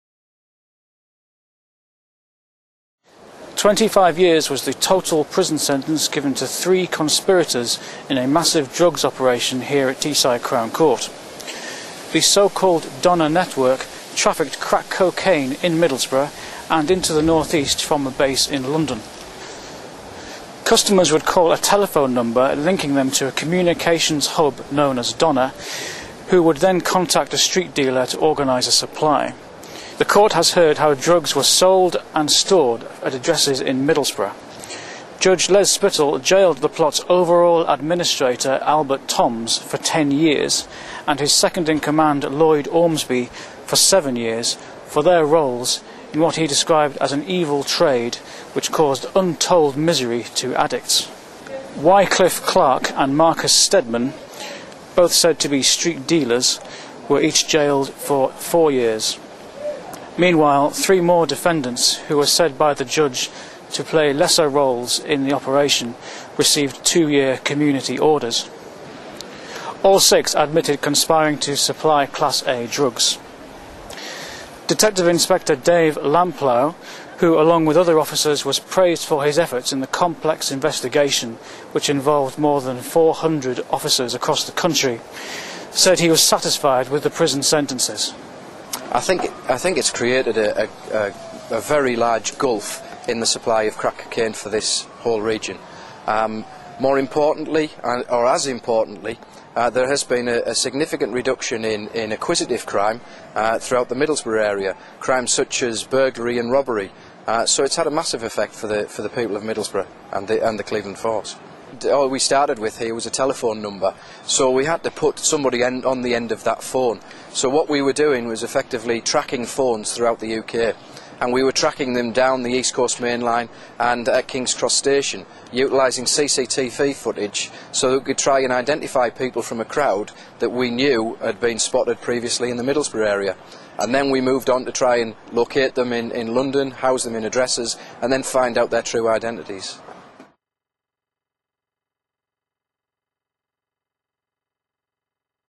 Drugs network jailed on Teesside
Click play for a video report on the jailing of a massive national crack cocaine network.